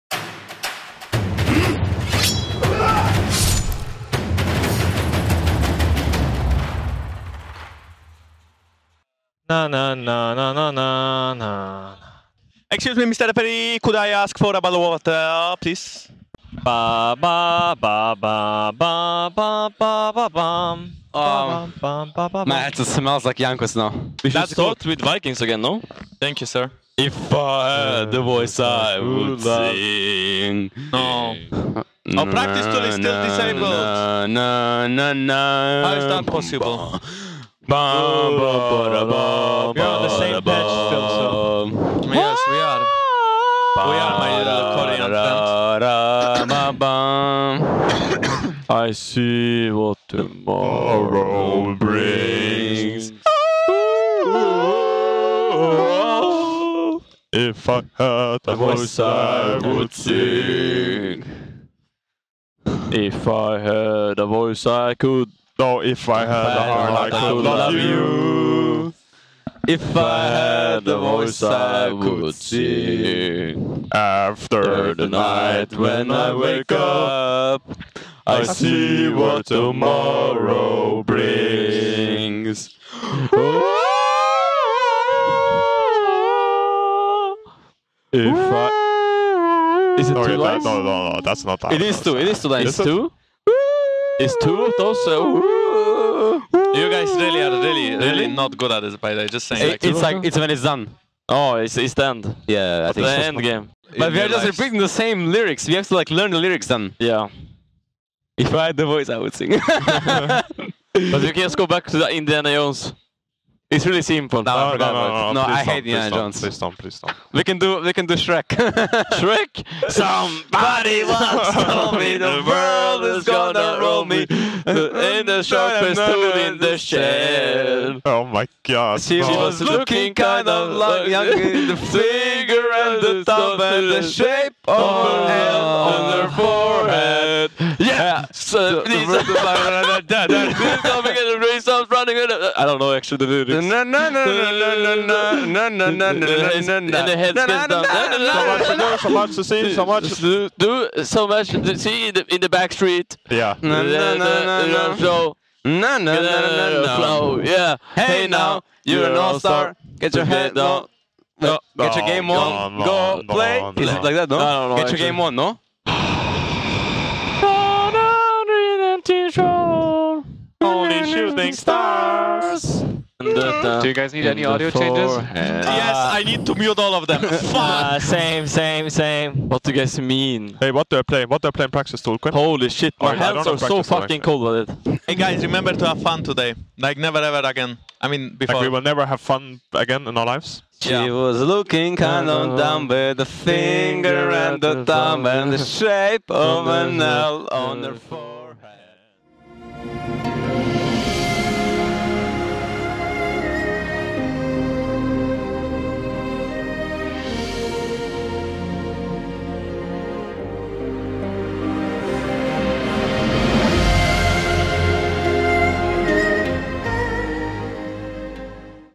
G2 Esports: LCS Waiting Room | EU LCS Voice Comms
With their heavenly (or hellish) voices, who needs Muzak when we've got Wunder, Perkz, and Hjarnan? Listen to our League of Legends team hype themselves up as they wait to get into game.